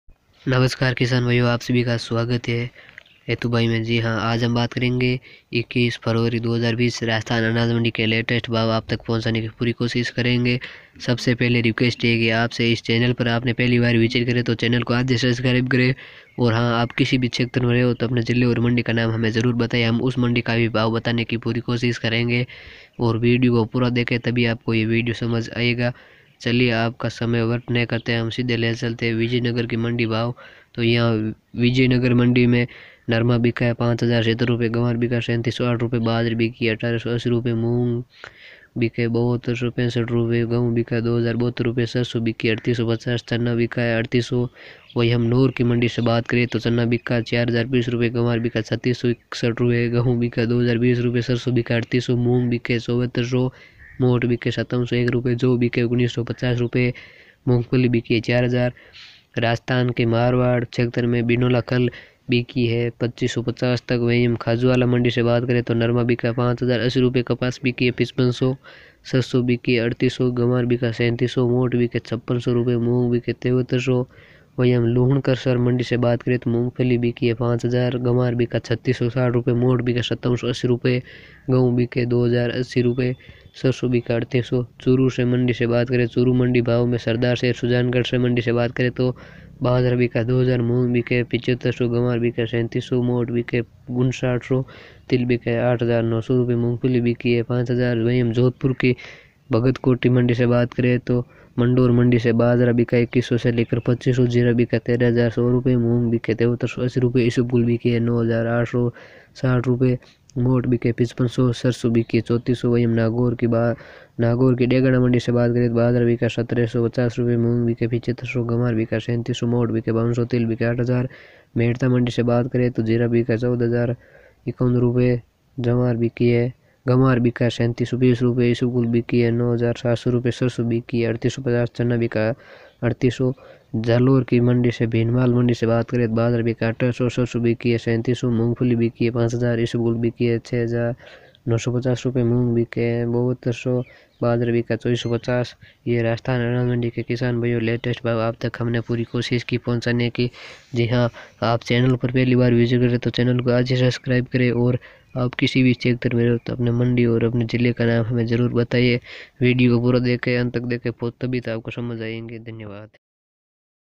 आज 21 फरवरी 2020 राजस्थान अनाज मंडी ताजा भाव नागौर बीकानेर जोधपुर चूरू गंगानगर हनुमानगढ़ Mandi retes
नमस्कार किसान भाइयो आप सभी का स्वागत है हेतु भाई मे ।।

आज 21 फरवरी 2020 राजस्थान अनाज मंडी ताजा भाव ।।।

नागौर बीकानेर जोधपुर बाडमेर चूरू गंगानगर हनुमानगढ़ अलवर सीकर जालोर समेत सम्पूर्ण राजस्थान अनाज मंडी ताजा भाव। ।।

मूंग मोट ग्वार ज्वार सरसो गेहूं मुगफली इसबगोल कपास सरसो रायडा मैथी ज्वार सभी अनाज मंडी ताजा भाव